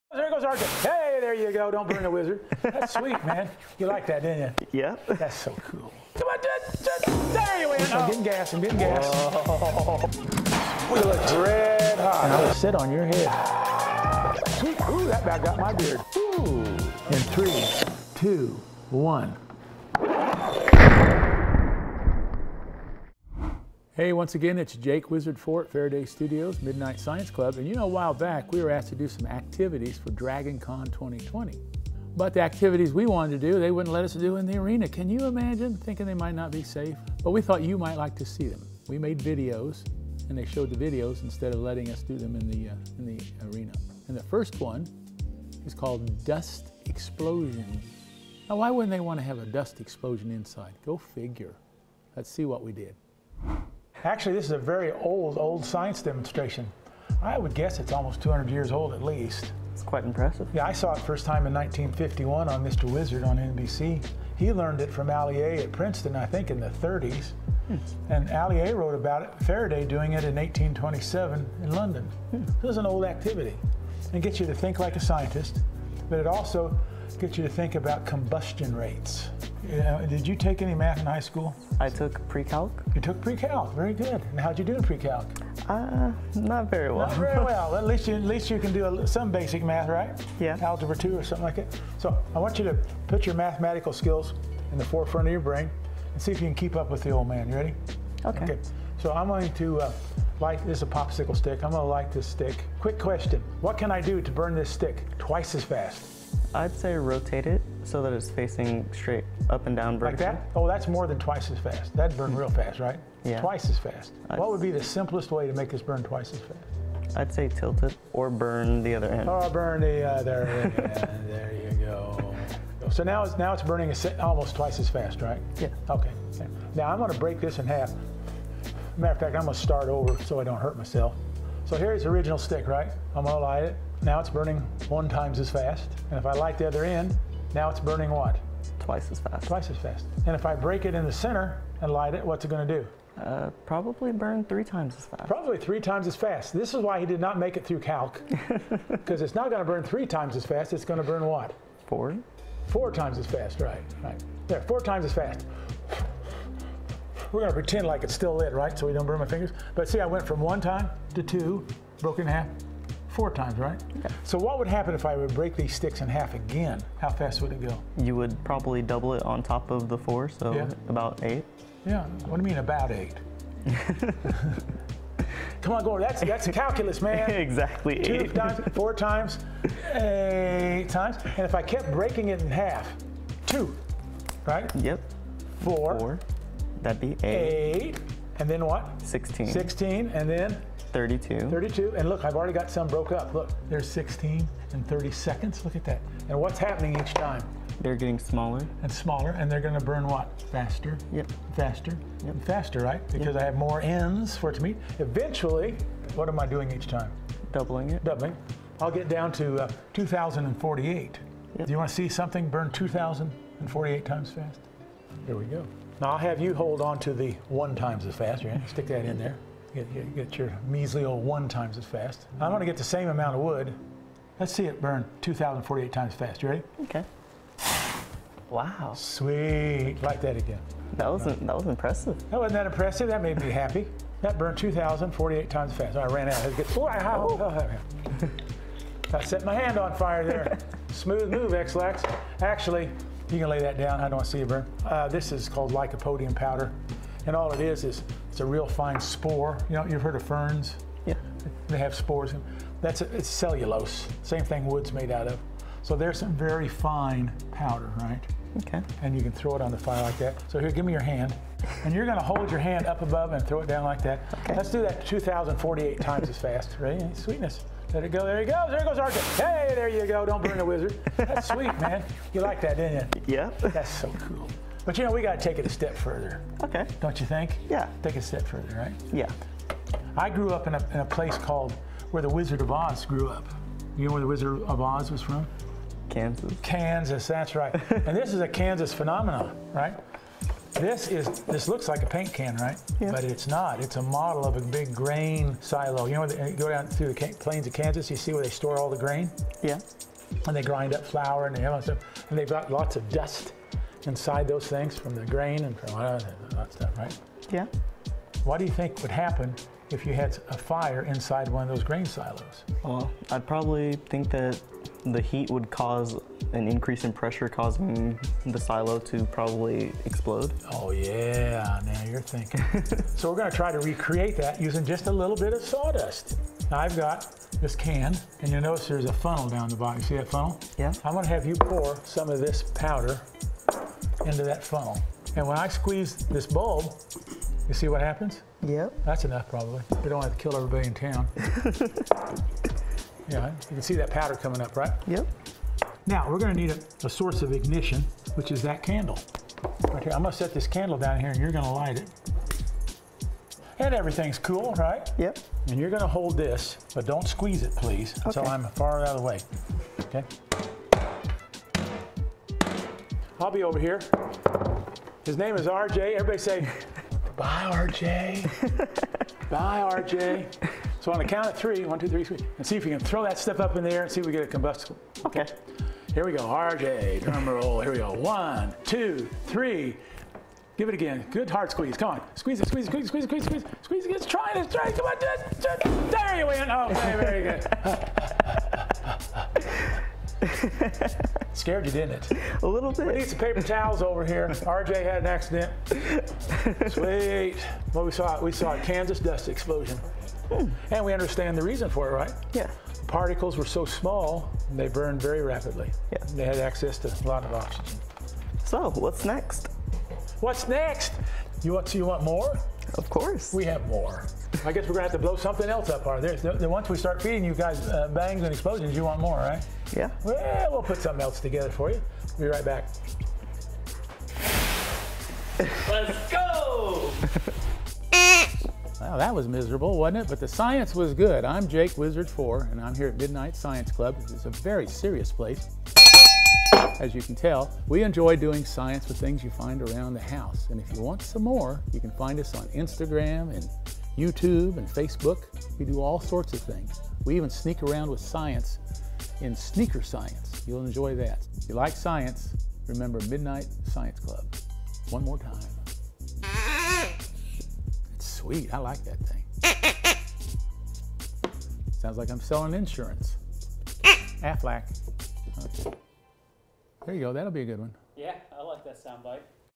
Dragon's Breath // Science Demo
We love a fiery science demo! This "dust explosion" is actually a very old science demonstration dating back to the 19th century. Learn about combustion – and practice your simple math – with this video.

This science demo was conducted in a safe environment, with expert supervision - don't try this one casually at home! Instead, enjoy the "whoooosh!" remotely...

Want to see content like this early, get behind the scenes access, and be a part of future videos?